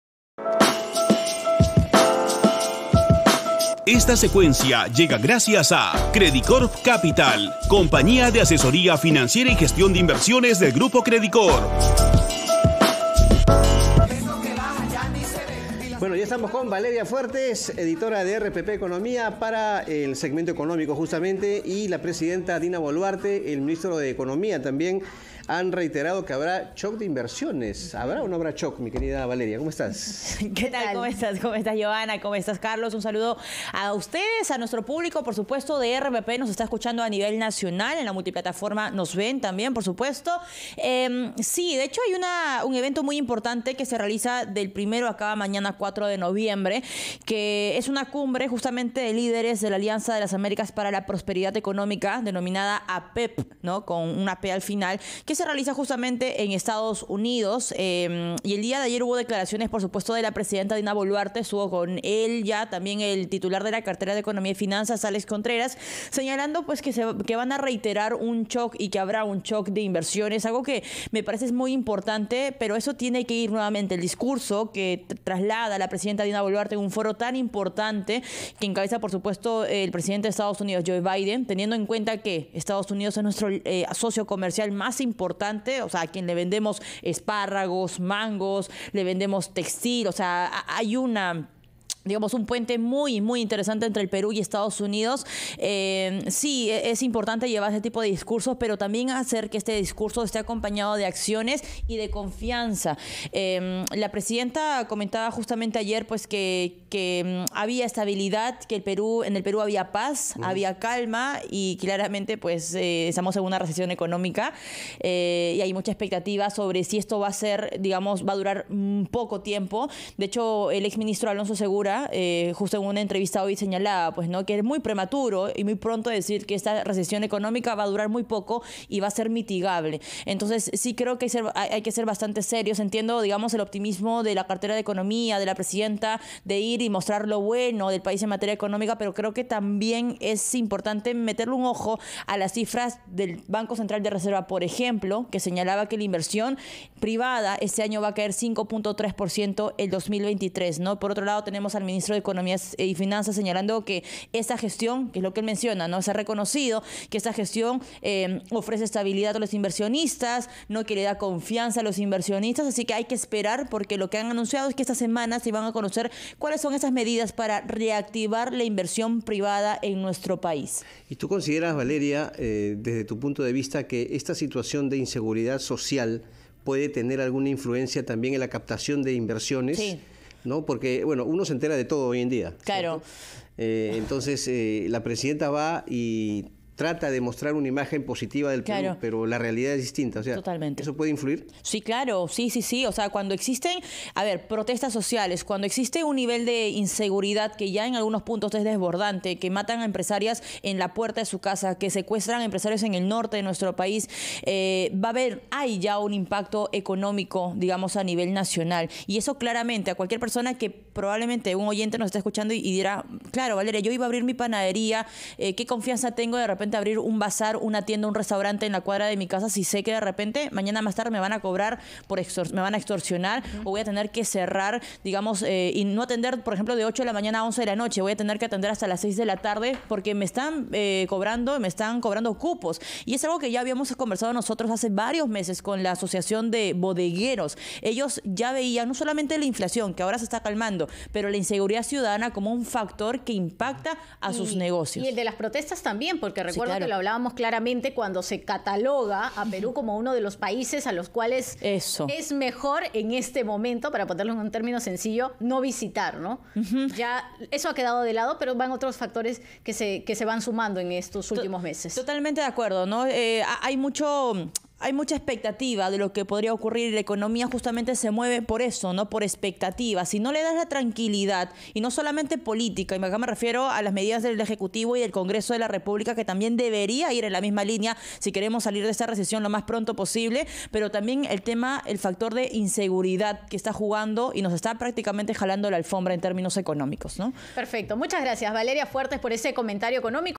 Gobierno anuncia la implementación de un shock de inversión
🗣️ La presidenta Dina Boluarte y el ministro de Economía anunciaron que habrá un shock de inversión. ¿Qué significa y cómo nos afecta? Valeria Fuertes, editora RPP Economía, explica más de esta medida.